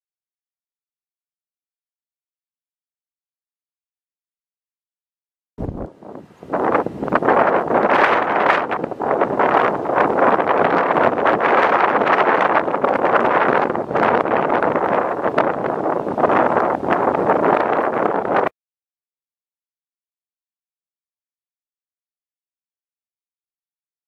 Iron Horse, tug boat, April 25, 2017
Quality Towing
From Foreshore Park on the banks of the North Arm of the Fraser River Burnaby, BC., Canada